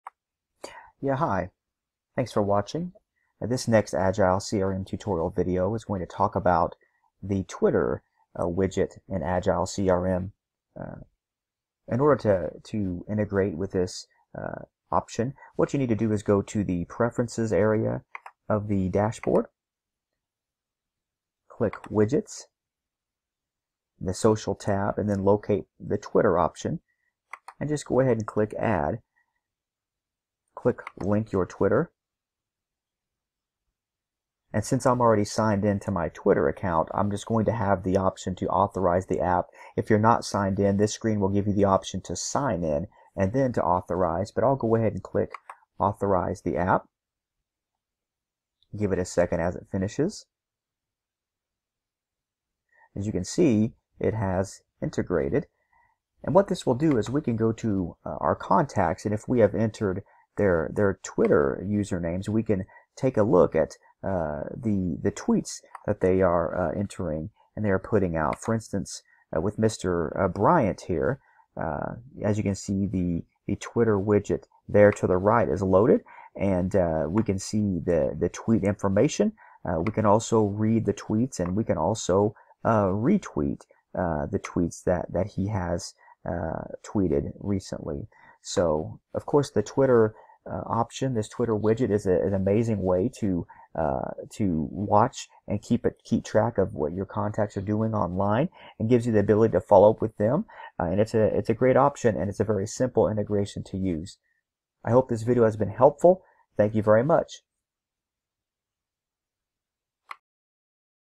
Twitter Widget - Agile CRM
Twitter Widget in Agile CRM allows you to monitor your customers social profile, their interests and gives you a chance to tweet to them directly from the widget .For more info on Twitter integration in Agile CRM:

Try Agile CRM today and enjoy the power of sales and marketing tools in one system: